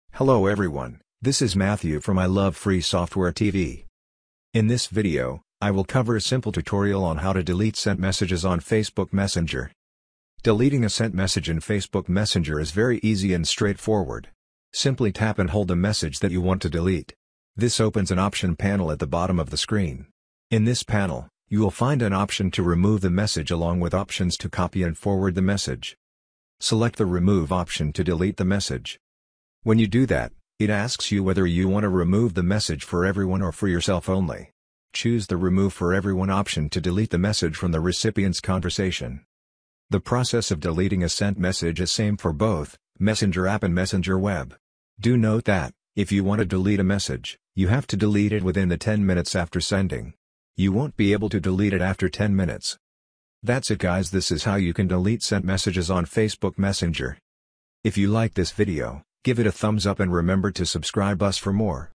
How To Delete Sent Messages on Facebook Messenger
Deleting a sent message in Facebook Messenger is very easy and straightforward. Simply tap and hold the message that you want to delete. This opens an option-panel at the bottom of the screen. In this panel, you will find an option to remove the message along with options to copy and forward the message.

Select the remove option to delete the message.

When you do that, it asks you whether you want to remove the message for everyone or for yourself only. Choose the "Remove for Everyone" option to delete the message from the recipient's conversation.

The process of deleting a sent message is same for both, Messenger App and Messenger Web. Do note that, if you want to delete a message, you have to delete it within the 10 minutes after sending. You won't be able to delete it after 10 minutes.

{ optional
Once you deleted the message, in the conversion, the message is replaced by a text notifying that this message is deleted although no one will be able to know what the message was; unless they read it prior to the delete.}